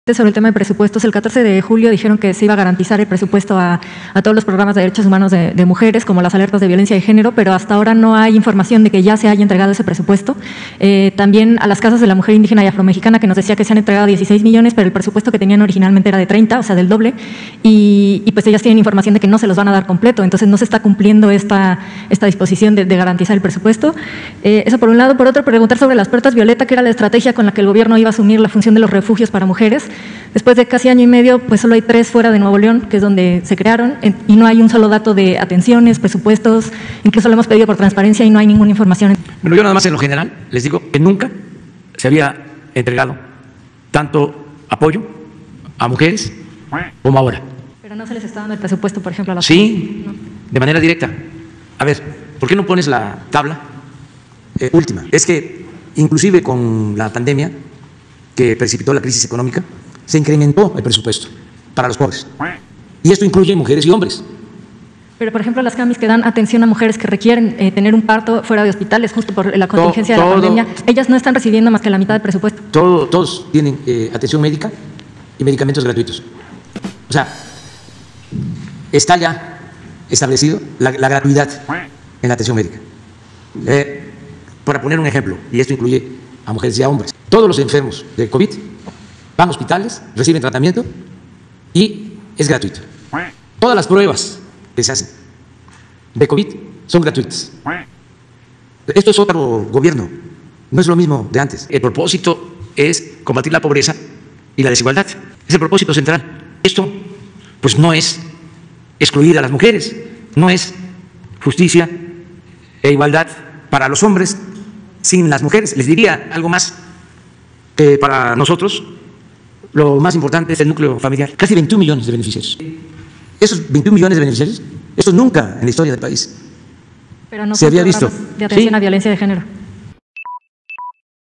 Sobre el presupuesto a programas que atienden a las mujeres
AMLO insiste que sus programas clientelares del bienestar son la solución para atender la agenda de género, incluisive “ya se estableció la gratuidad en atención médica”, “todas las puebas de covid son gratuitas” 🦆